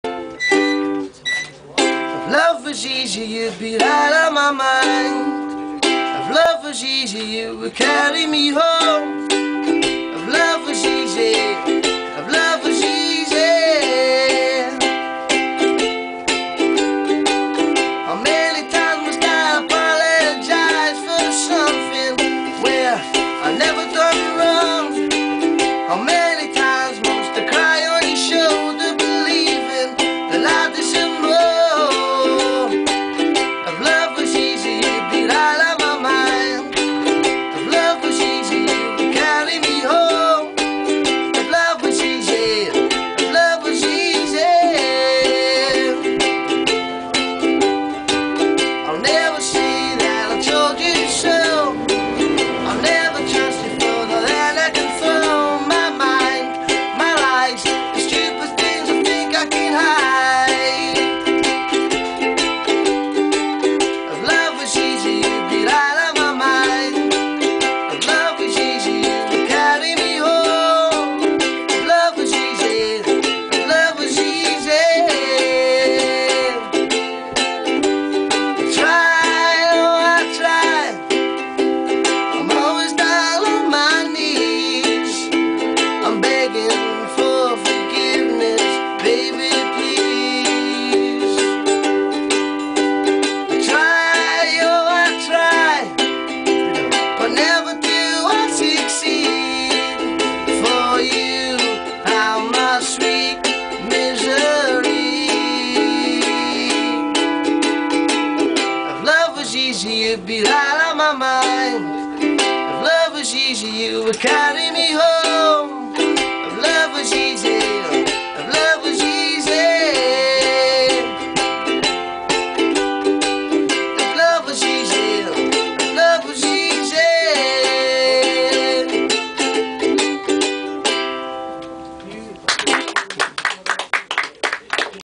Billy Mitchell @ Dundee Jams for Oxjam instore gigs, 1/10/11. Movie by Daisy Dundee
Billy performing a new song from his band The Twist in a Dundee shop window as part of Charity Begins at Home.